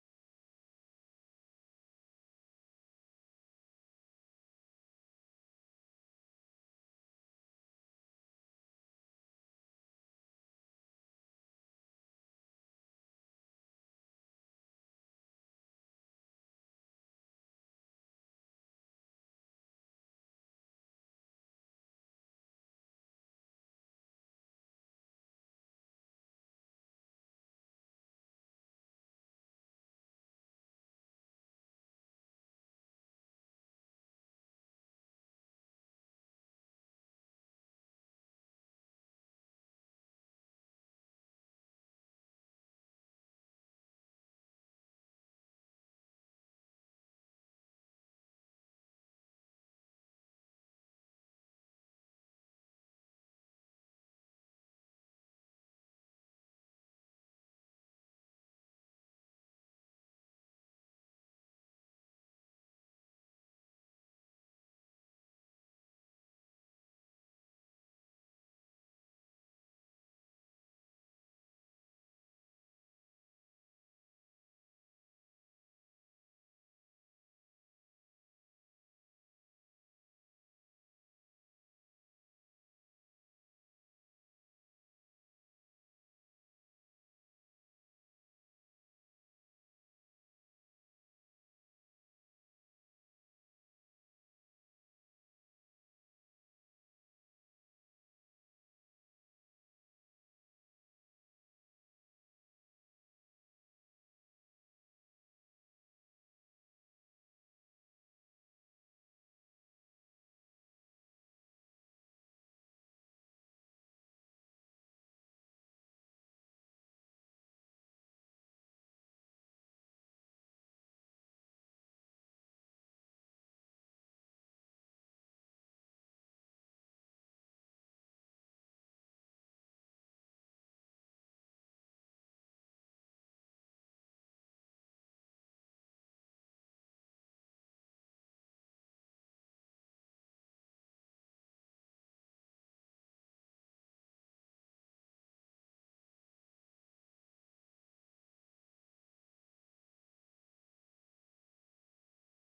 Guitar thrashing
sorry guys, it was muted wen it was recording so u cant hear wat we were saying:( sorry, but still it luks funny. We were so freakin HYPER!